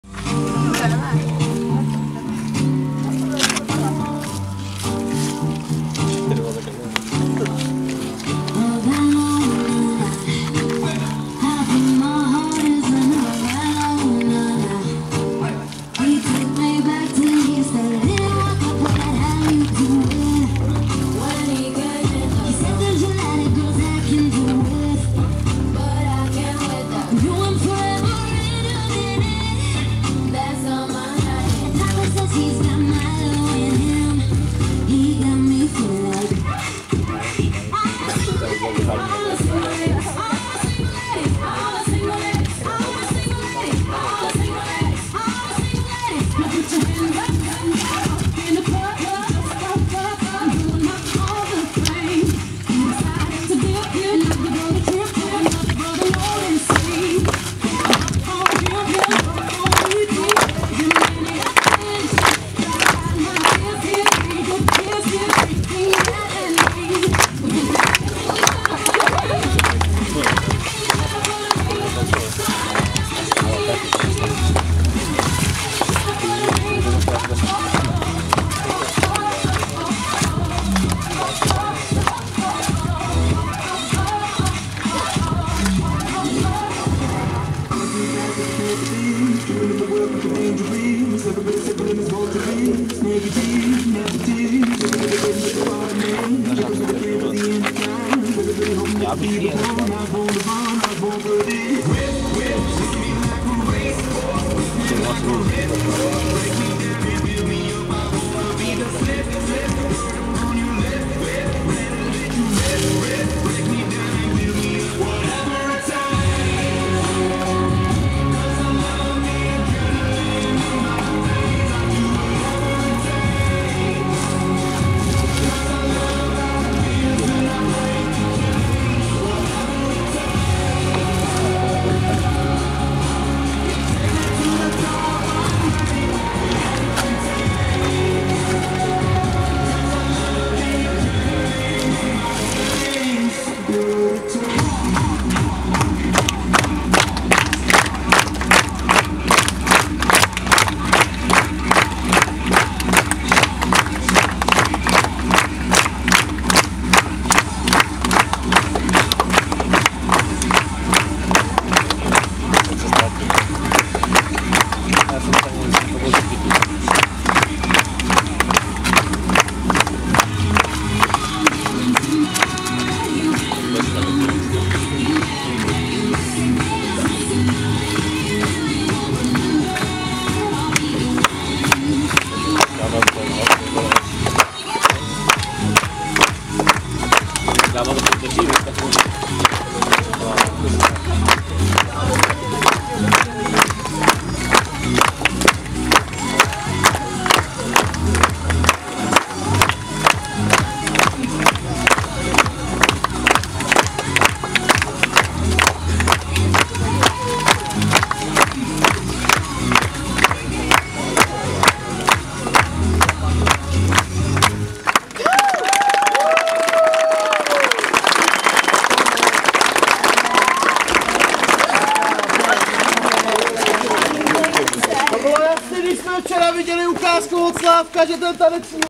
Tábor Blaník 2018 - I.Turnus 2018
Taneční vystoupení Tanečního oddílu na I.Turnusu.